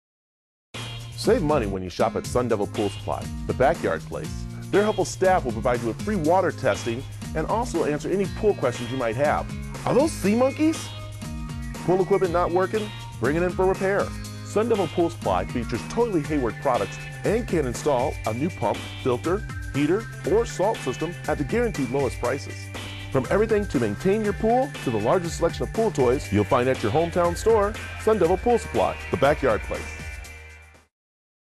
Brad Perry for Sun Devil Pool Supply
Brad Perry's new commercial for Sun Devil Pool Supply in Phoenix